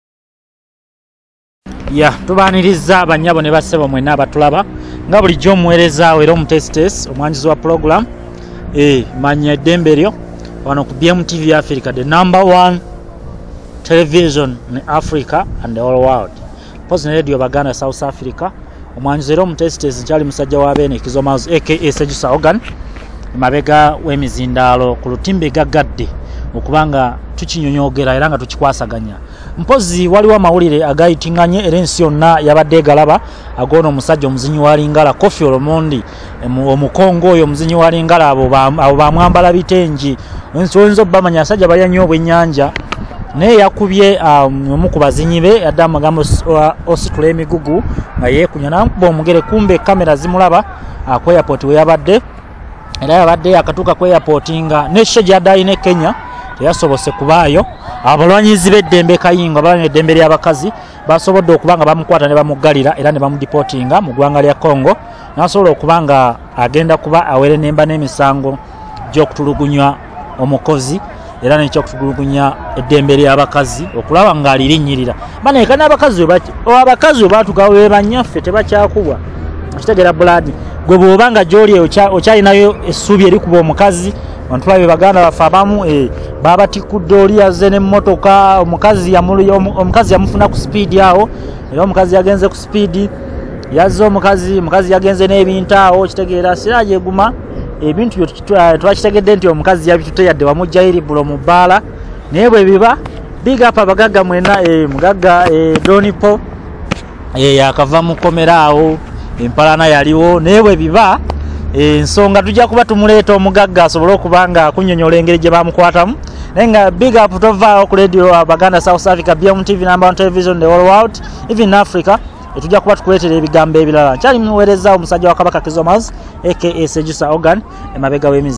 Omuzaire KOFFI OLOMOIDE afuntude omuzinyi ensambagere ,Kenya ne mudepotinga
SEJUSA MILES Spoils the Soup... Koffi Olomide case: Kenya deports singer over airport 'kick'